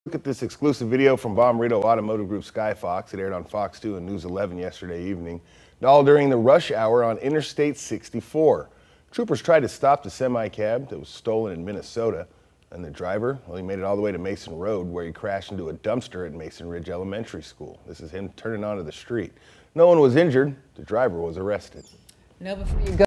Police chase stolen semi-truck in St. Louis County
A man was taken into custody Tuesday evening after stealing a semi-truck.

According to a spokesperson for the Missouri State Highway Patrol, the vehicle was stolen out of Minnesota, and the truck company had tracked it by GPS.

Officers received a call just after 4 p.m. about the stolen vehicle. They had located the semi on Interstate 64 near Highway K and tried to stop the driver, but he refused to pull over, which then led to a chase. The pursuit’s speed varied from 20 to 75 miles per hour.

Top story: Police officer’s gun and body armor stolen from his car
Officers caught up with the suspect around St. Charles County; he then headed east into St. Louis County. The spokesperson said the driver then jumped out of the semi near Mason Ridge, and the truck rolled into a dumpster at Mason Ridge Elementary School to come to a stop.

The driver fled on foot, running around the school before Highway Patrol troopers caught up with him and arrested him.

No one was injured during the pursuit.

Police said the 31-year-old suspect is from Minnesota and is currently in a St. Charles County jail awaiting charges.